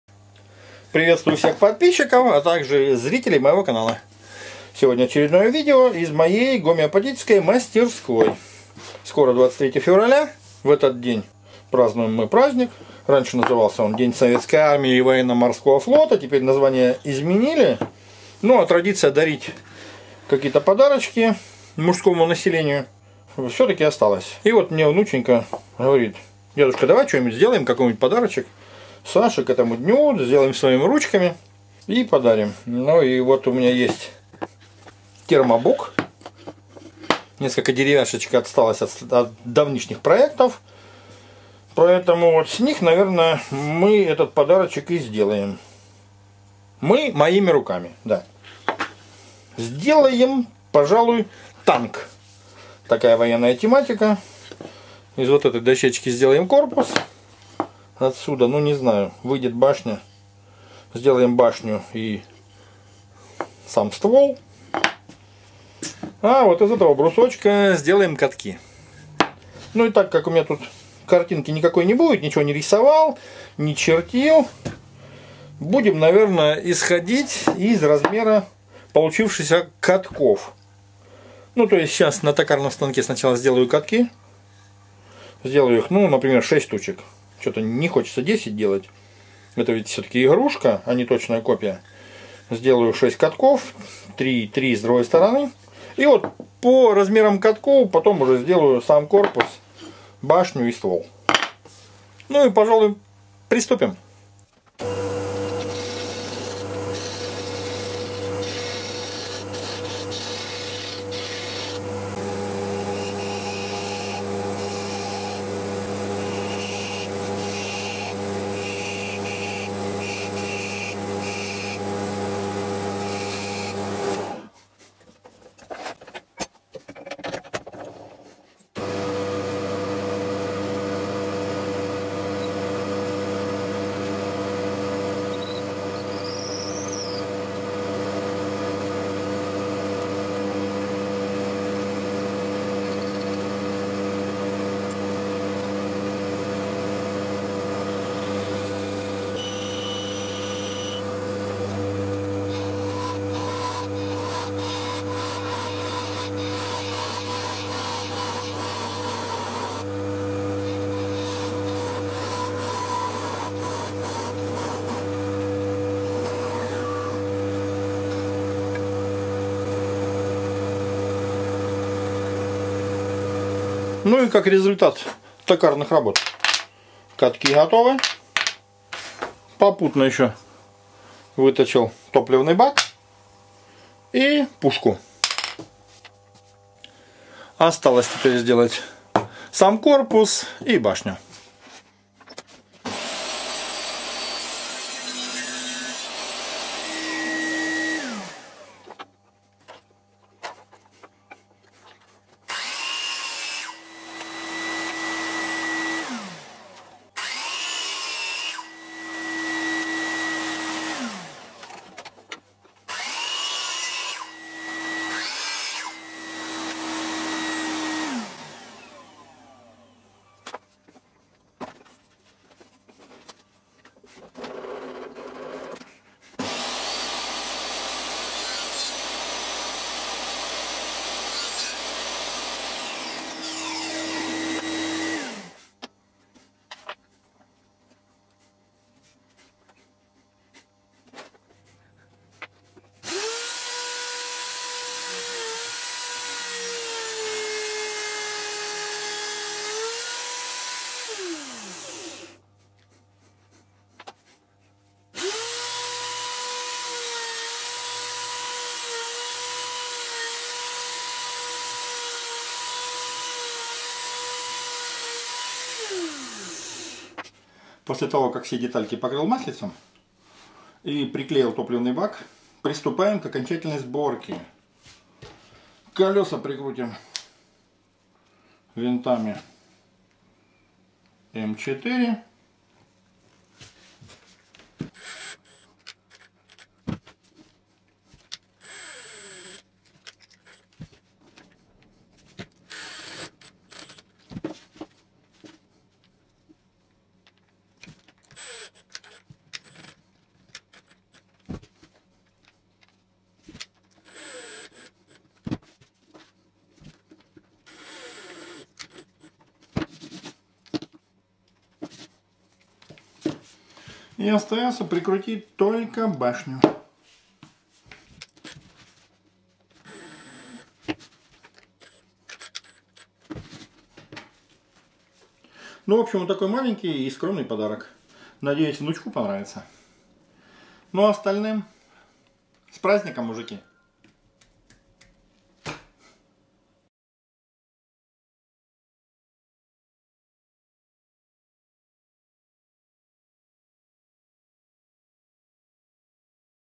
Как сделать танк -игрушку из дерева. Мастер-класс
Игрушки, сделанные из дерева, являются не только экологически чистыми, но и достаточно долговечными, если сравнивать их с пластмассовыми.
По совету внучки решил сделать деревянную игрушку в подарок моему внуку ко Дню Защитника Отечества. Решили с ней вместе, что это будет танк, ну а как всё получилось можно посмотреть в данном ролике